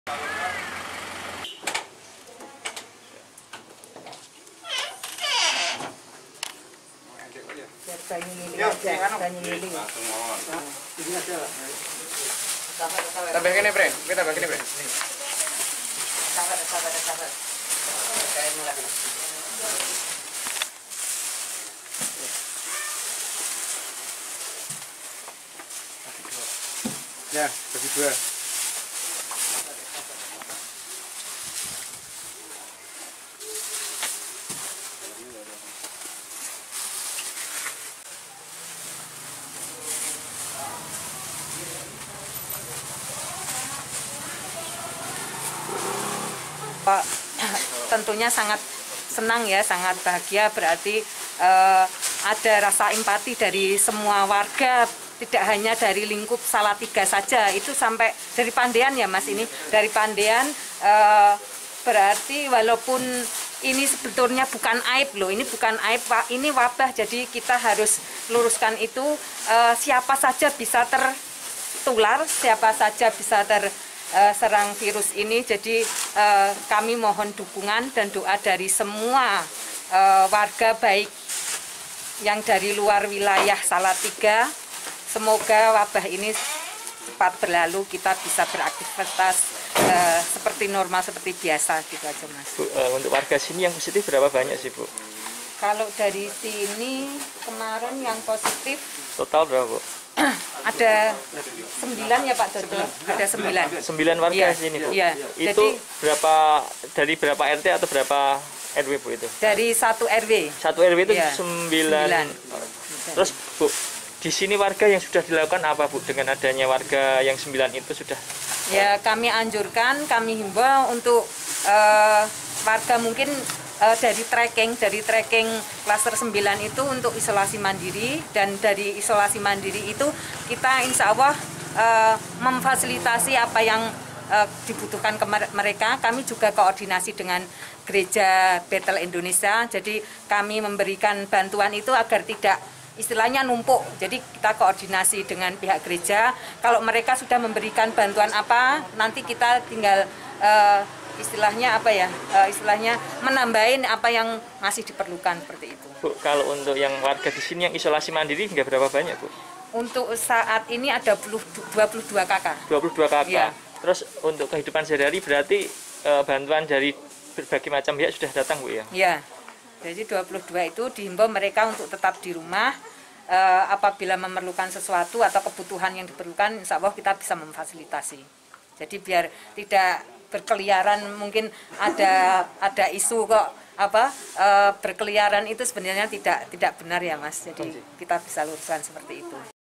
TIDAK HARUS UANG DAN BERAS, PETANI MAGELANG SUMBANG SAYURAN KE WARGA SALATIGA LHO
Apapun itu yang penting ikhlas. Kalian punya apa? Jangan lupa sedekah ya?